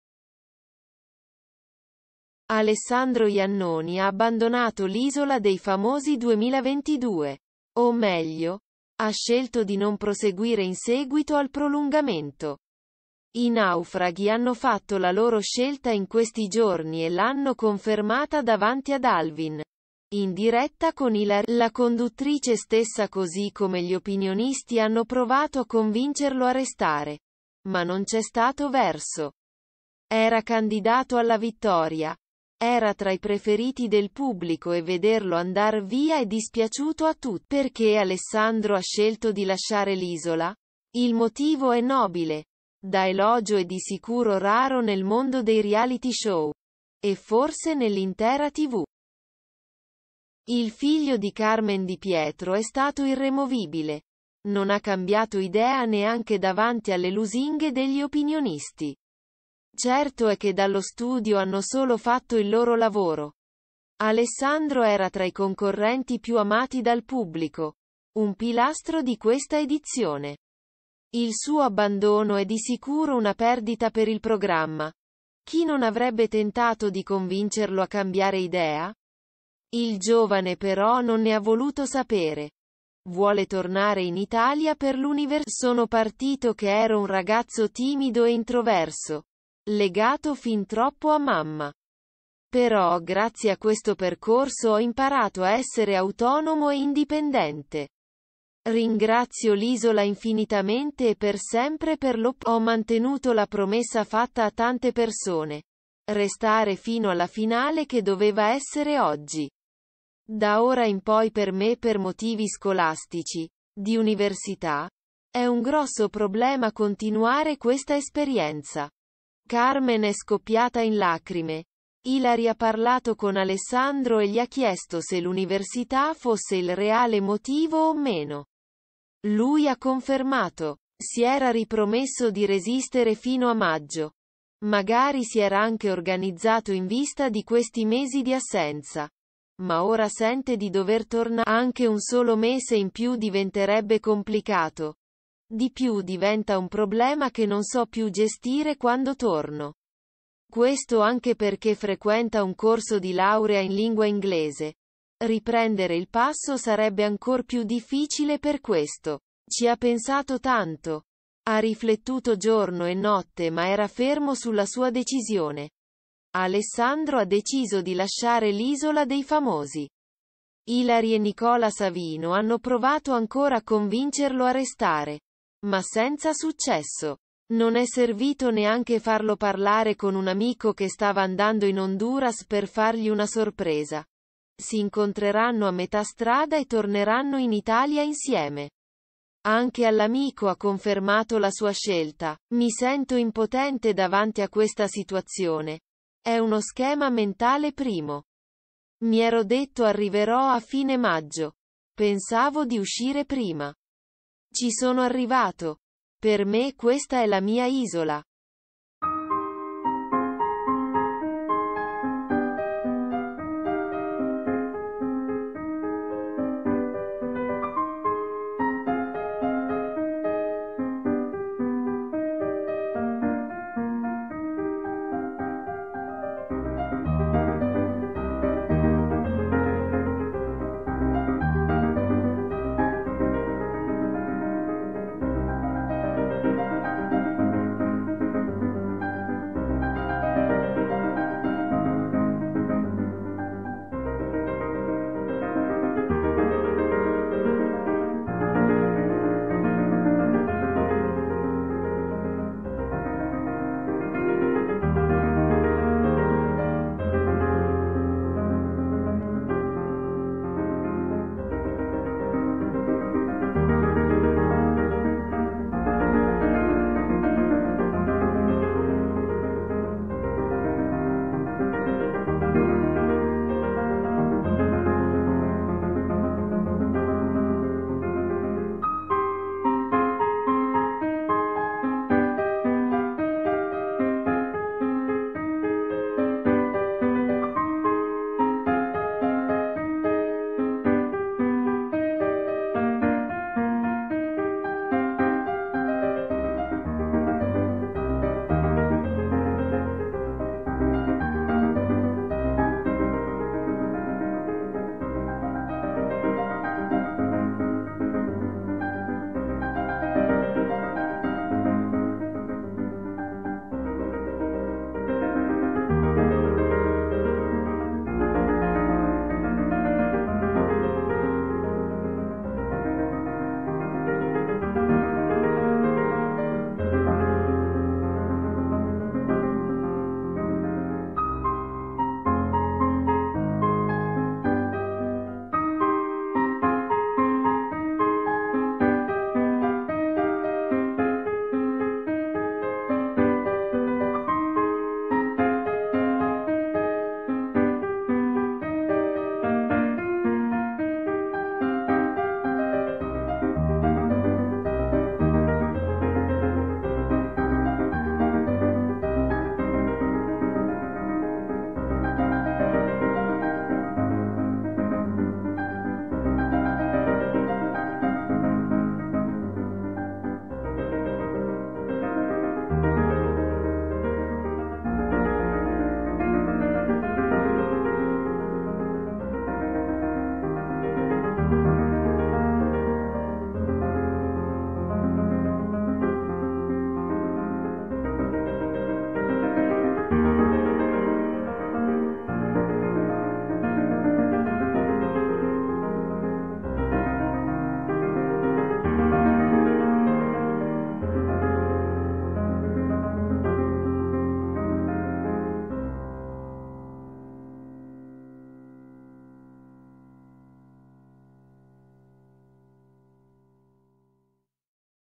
Isola 2022, Alessandro Iannoni ha abbandonato: il motivo, elogi in studio
Triste colpo per il pubblico del reality show: il figlio di Carmen Di Pietro era tra i preferiti, e pure tra i favoriti alla vittoria